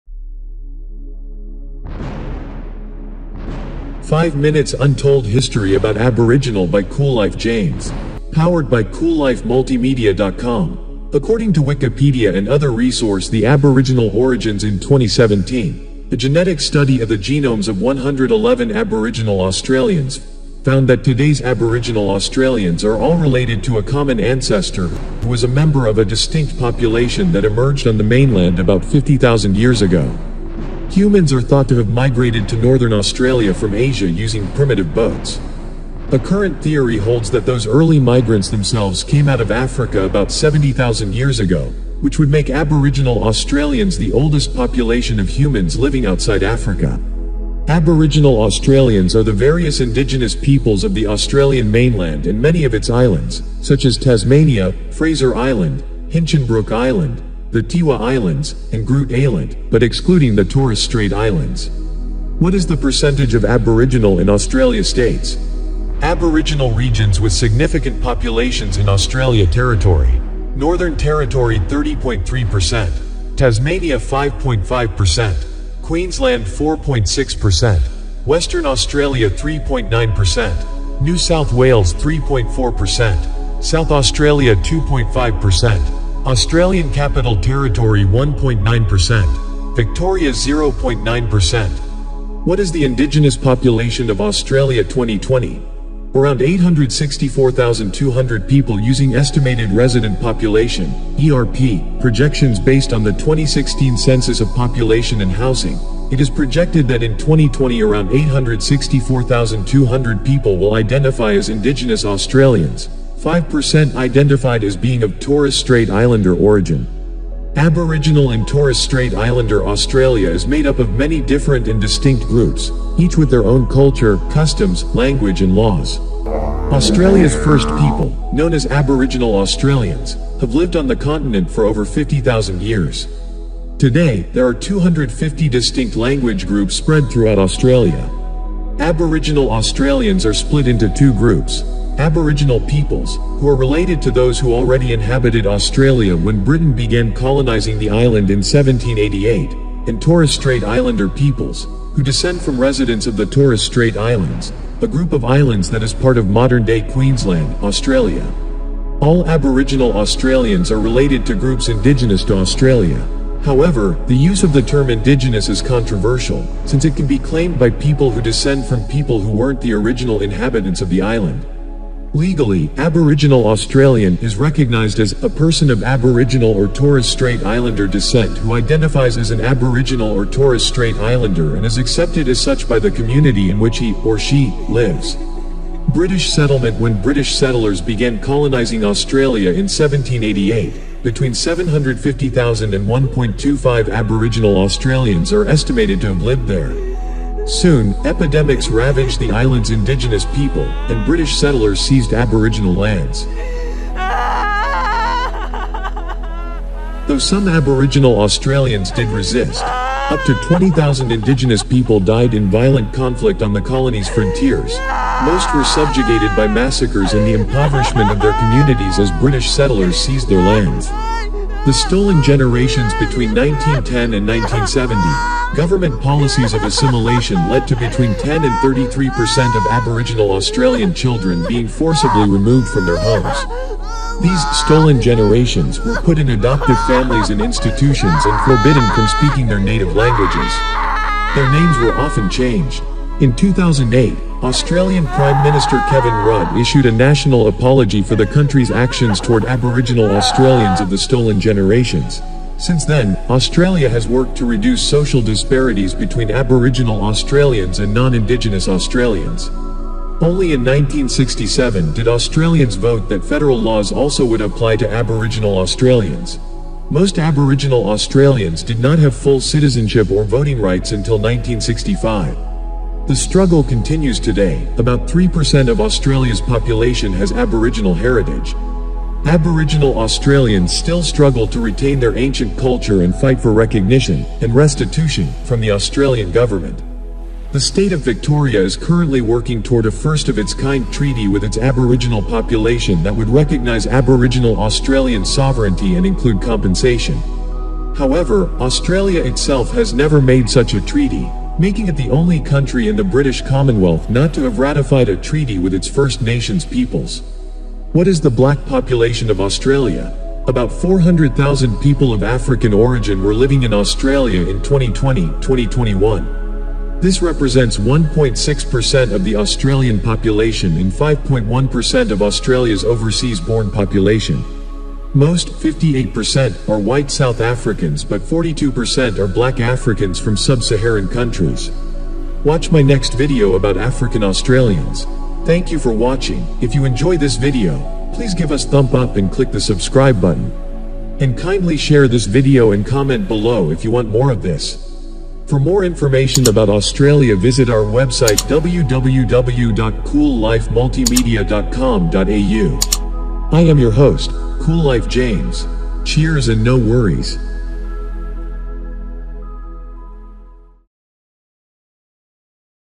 Aboriginal Untold History -The Stolen Generation - Documentary By Koollife James Kolawole
Aboriginal Untold History -The Stolen Generation By Koollife James Powered by Koollife Multimedia.

5 MINS Untold History about Aboriginal by Koollife James. Powered by Koollife Multimedia.com.

according to Wikipedia and other resource
The Aboriginal origins In 2017, a genetic study of the genomes of 111 Aboriginal Australians found that today’s Aboriginal Australians are all related to a common ancestor who was a member of a distinct population that emerged on the mainland about 50,000 years ago. Humans are thought to have migrated to Northern Australia from Asia using primitive boats. A current theory holds that those early migrants themselves came out of Africa about 70,000 years ago, which would make Aboriginal Australians the oldest population of humans living outside Africa.

Aboriginal Australians are the various Indigenous peoples of the Australian mainland and many of its islands, such as Tasmania, Fraser Island, Hinchinbrook Island, the Tiwi Islands, and Groote Eylandt, but excluding the Torres Strait Islands.

Aboriginal Regions with significant populations in Australia.
NorthernTerritory 30.3%.
Tasmania 5.5%.
Queensland 4.6%.
WesternAustralia 3.9%.
NewSouthWales 3.4%.
SouthAustralia 2.5%.
AustralianCapitalTerritory 1.9%.
Victoria 0.9%.

What is the indigenous population of Australia 2020? around 864,200 people
Using estimated resident population (ERP) projections based on the 2016 Census of Population and Housing, it is projected that in 2020 around 864,200 people will identify as Indigenous Australians, 5% identified as being of Torres Strait Islander origin.

Aboriginal and TorresStrait Islander Australia is made up of many different and distinct groups, each with their own culture, customs, language and laws.